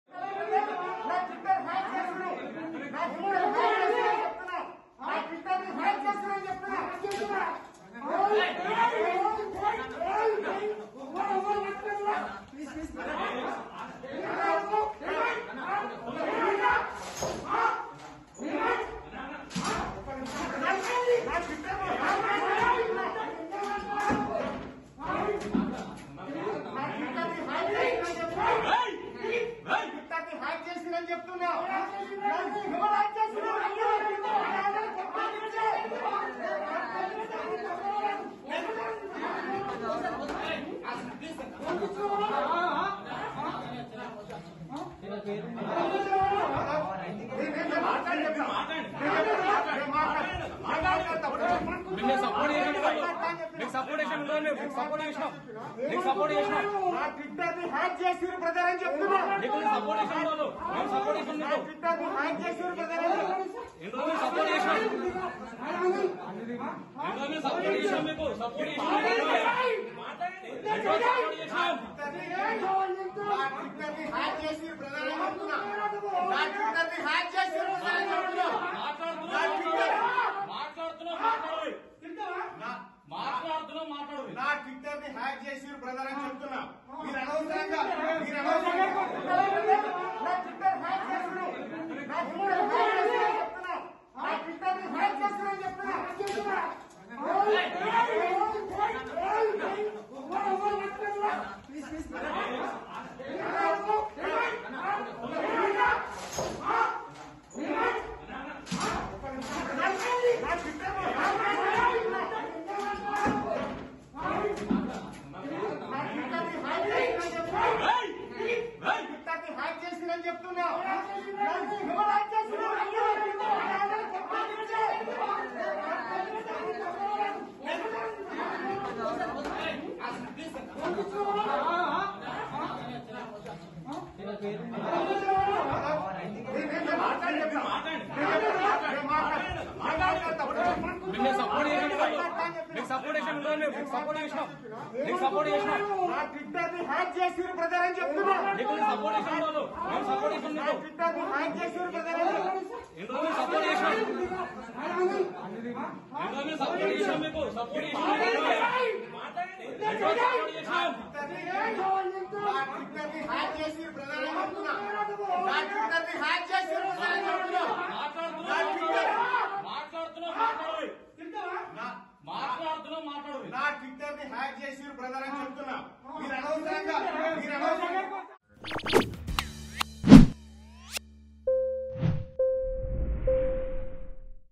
Teenmaar Mallanna : తీన్మార్ మల్లన్న పై గుర్తుతెలియని వ్యక్తుల దాడి I News18 Telugu
Teenmaar Mallanna : తీన్మార్ మల్లన్న పై గుర్తుతెలియని వ్యక్తుల దాడి